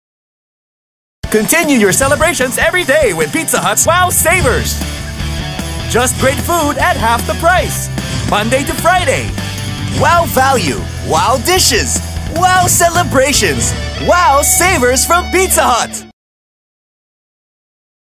Piza Hut Wow Savers TVC
Great Food, Half the Price! Just visit your favorite Pizza Hut restaurant from opening to 6pm from Mondays to Fridays*.

*excluding Holidays